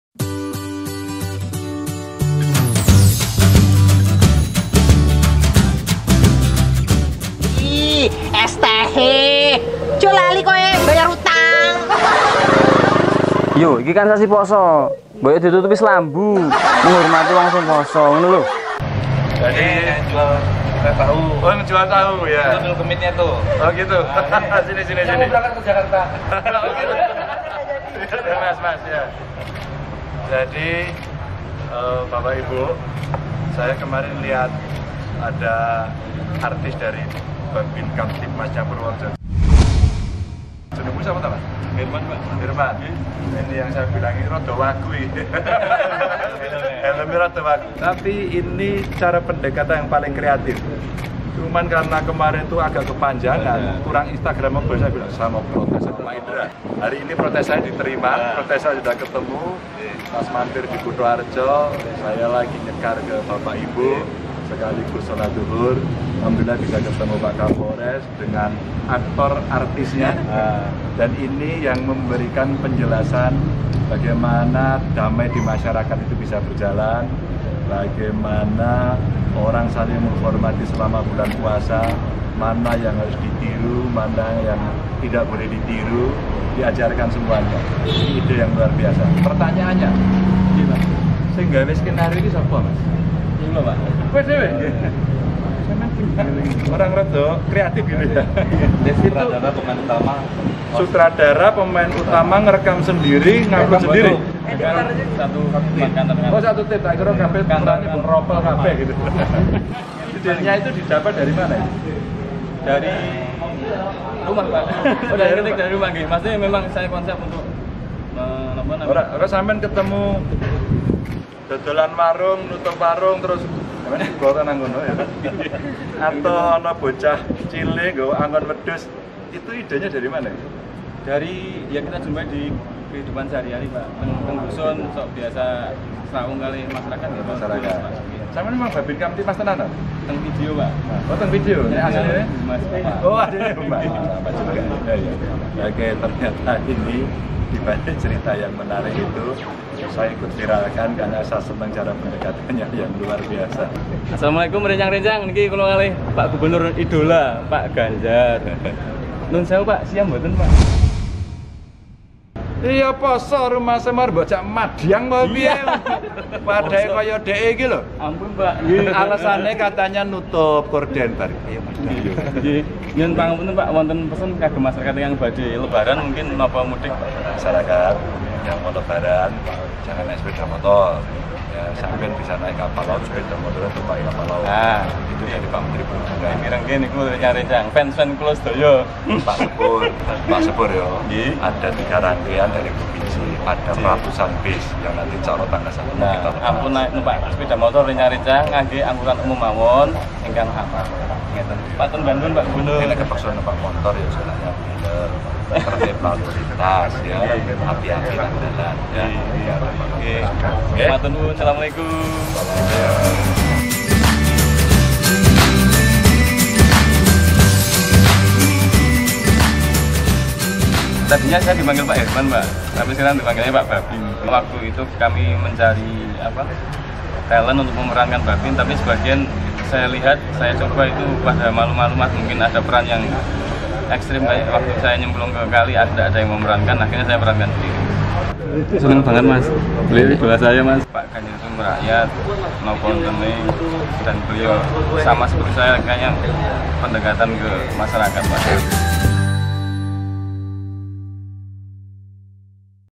Ganjar Pranowo : Sampeyan Rodo Wagu, Ning Kreatif - Ketemu Youtuber Bripka Herman ( Pak Bhabin )
Bripka Herman atau yang lebih luas dikenal dengan panggilan "Pak Bhabin (Bhabinkamtibmas)," memang kini tengah naik daun di jagat maya. Anggota Polres Purworejo itu sukses membuat masyarakat, khususnya netizen jatuh hati karena video-videonya yang kini viral, banyak memberikan hiburan lucu dan penuh pesan moral.

Salah satu fans dari Bripka Herman adalah Gubernur Jawa Tengah Ganjar Pranowo.

Bak gayung bersambut, ternyata Bripka Herman juga ngefans dengan Ganjar. Akhirnya, pertemuan keduanya terjadi di Masjid At-Taqwa, Daleman, Purworejo, Kamis (23/5/2019).